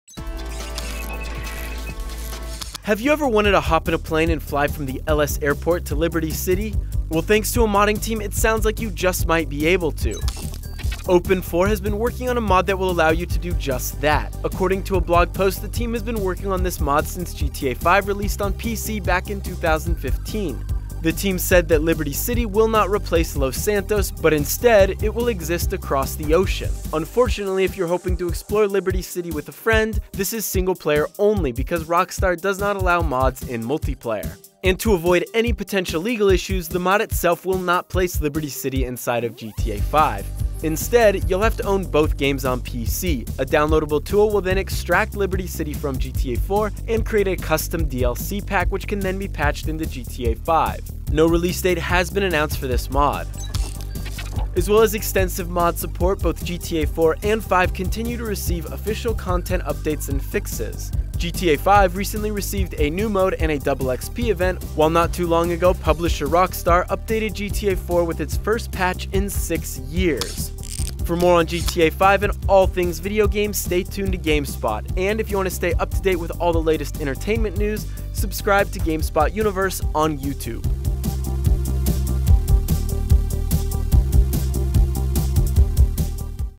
GTA 4's Liberty City Will Soon Be Playable within GTA 5 - GS News Update
Modders have brought Liberty City over to the PC version of Grand Theft Auto 5 using OpenIV.